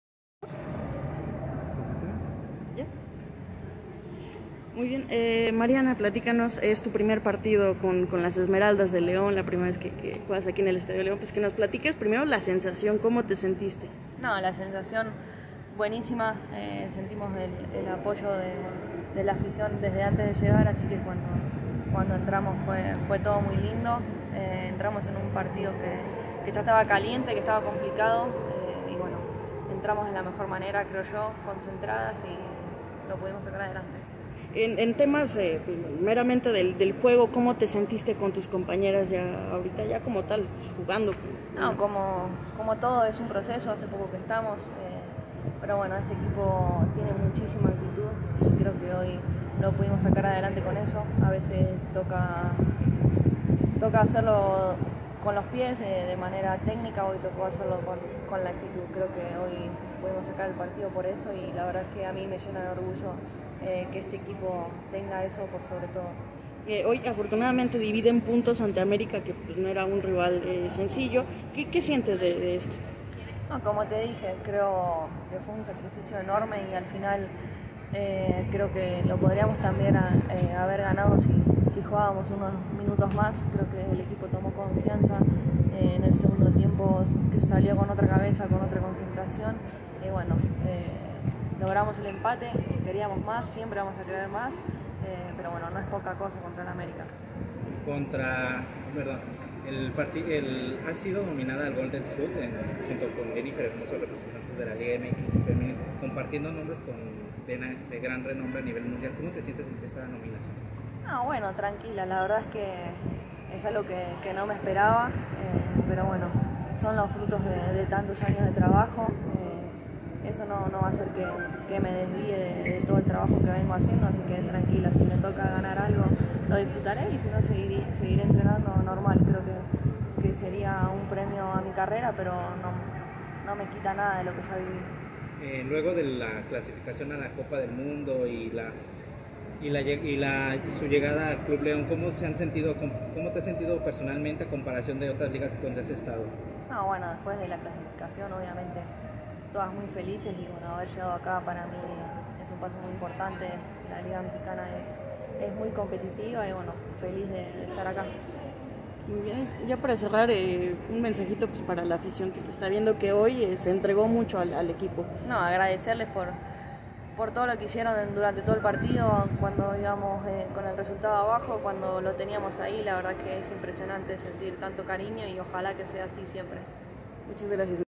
"Este equipo tiene muchísima actitud": Mariana Larroquette
Estas fueron las palabras de Mariana Larroquette tras debutar con el Club León en el empate 2-2 ante América.

🎥 Delia Juárez